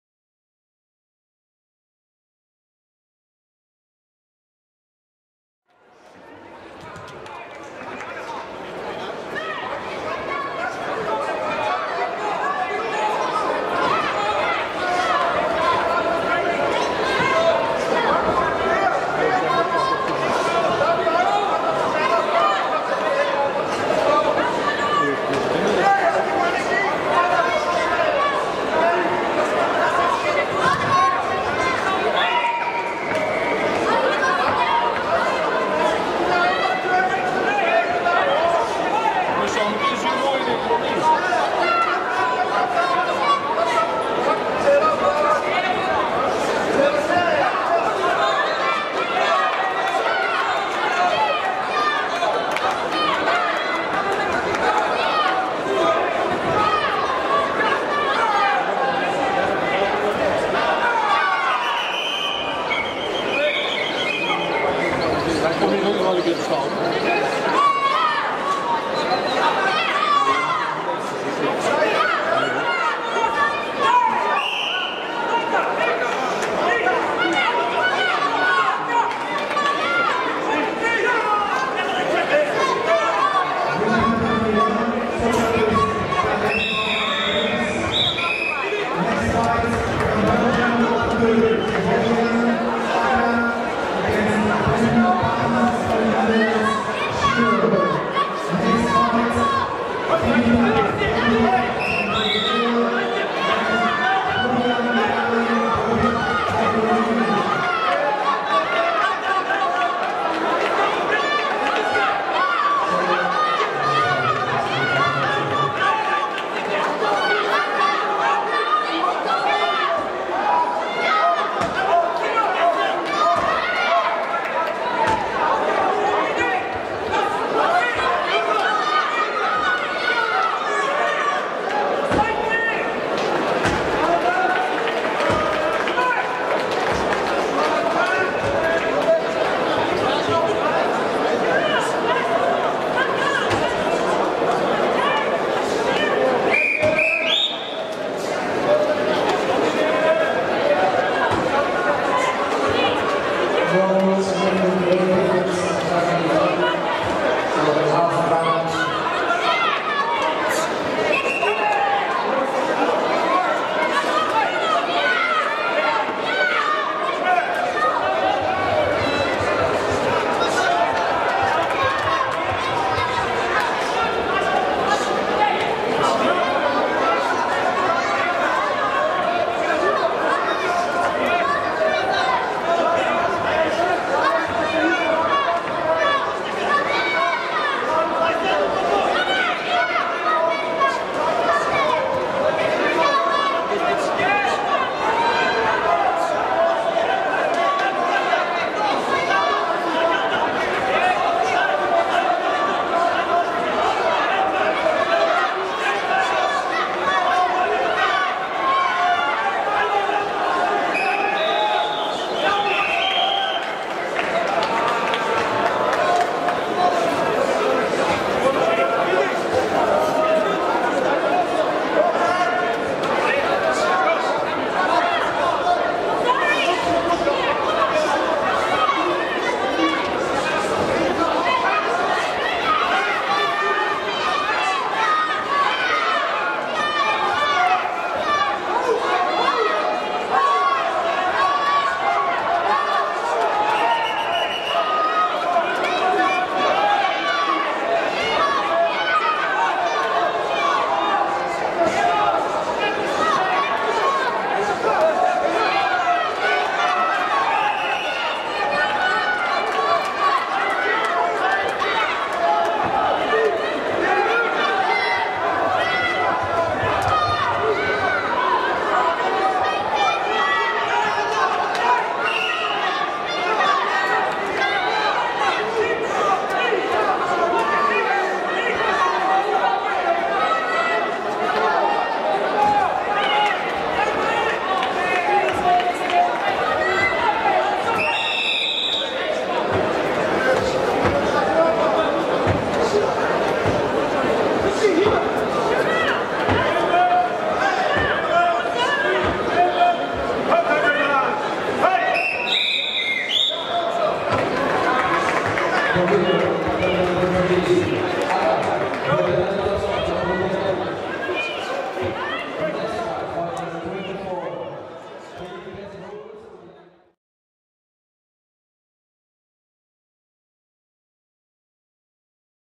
DiamondCup2018 Senior Women 57kgs Violeta LITOVSKA Bulgaria Vs Erica LY Netherlands
Diamond Cup 2018
Antwerpen - Belgium
24 février 2018